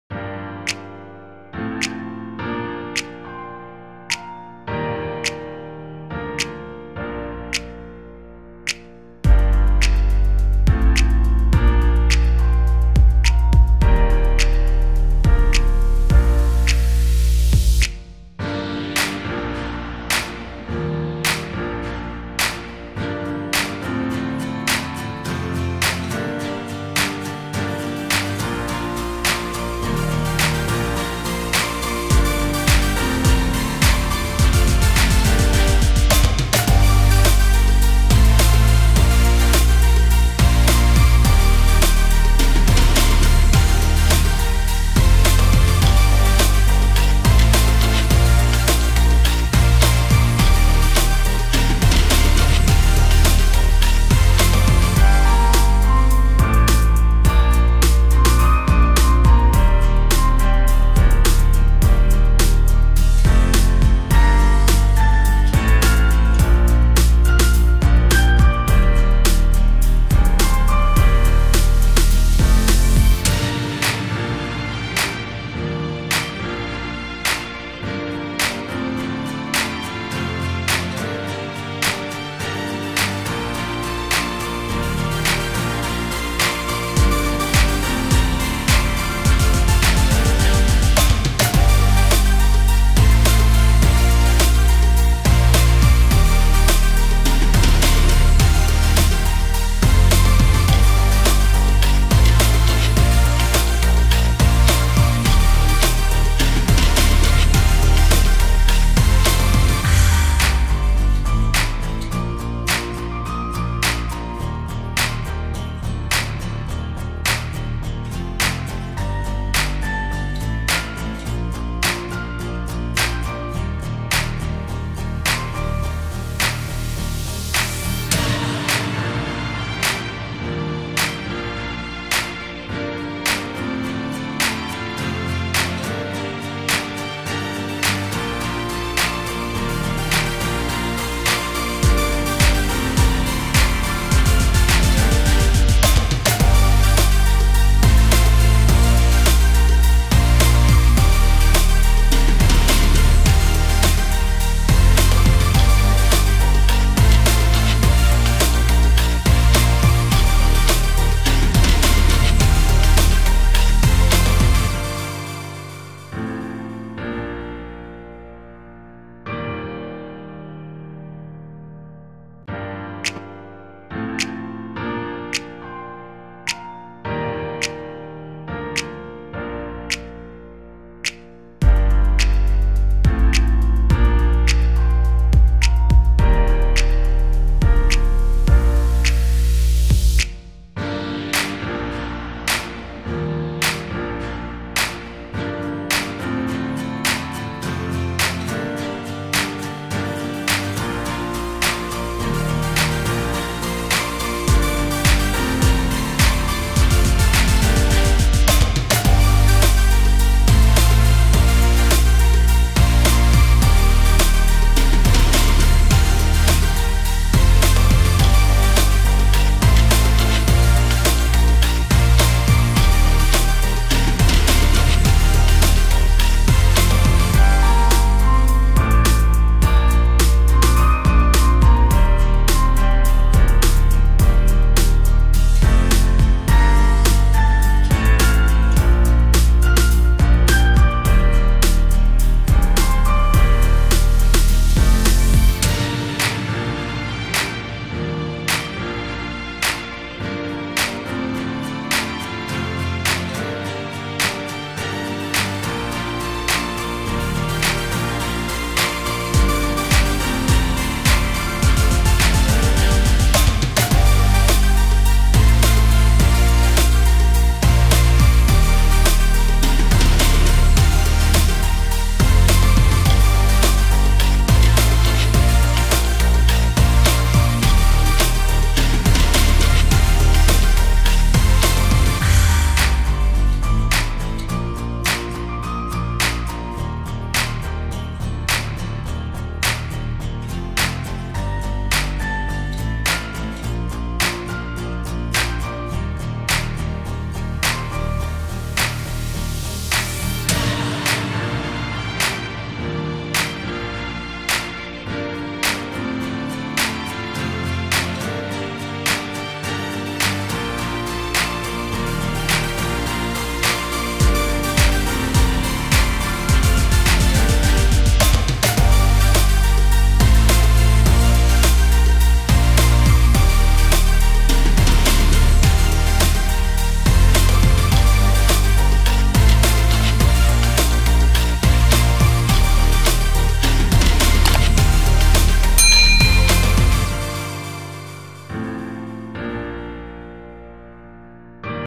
BUNGA MAWAR PITA SATIN / BUNGA LOVE PITA SATIN
Bunga mawar pita satin
Bunga love pita satin

Hai semua, kembali lagi dichannel @GaleriCantik  Di video ini saya membuat bunga love dari pita satin, tonton video tutorialnya sampai selesai ya, terima kasih.